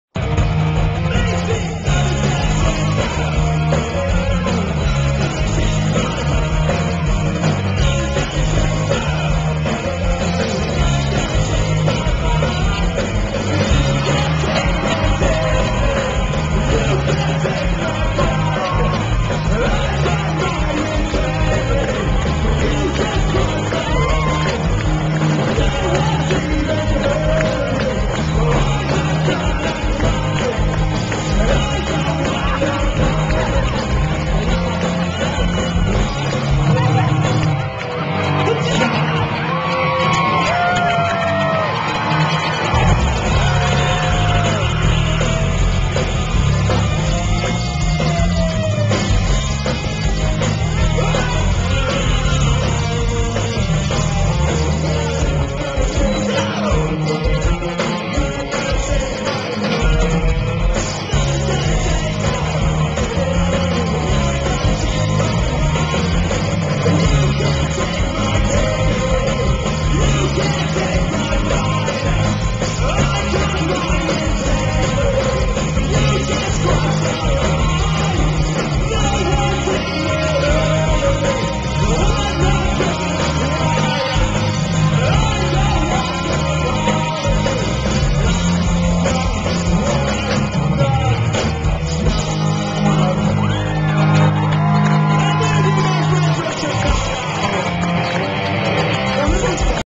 Hawk Nelson crowd surfing
Flip Video 11/15/09 at house of blues chicago